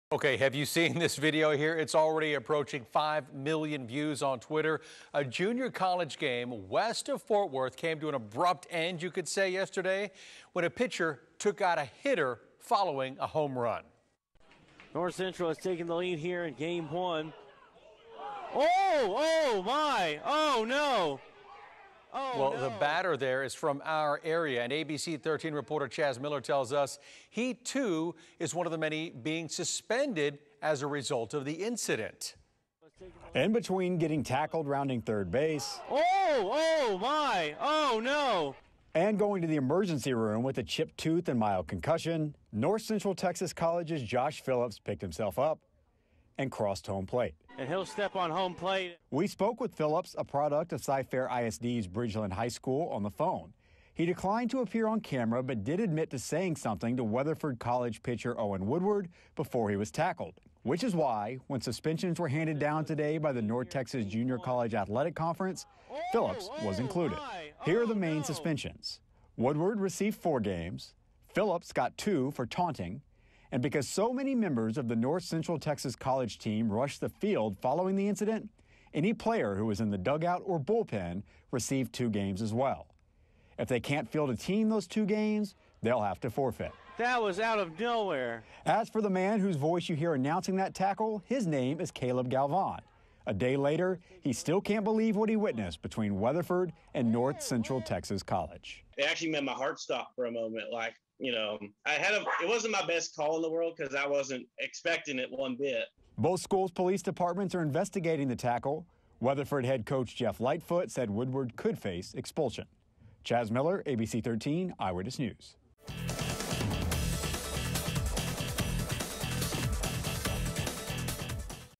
Multiple players suspended after Texas college pitcher's tackle on hitter that was caught on video
In the video, Josh Phillips, who is from the Cypress area, is seen rounding third base when pitcher Owen Woodward tackles him to the ground, causing his helmet to fly off.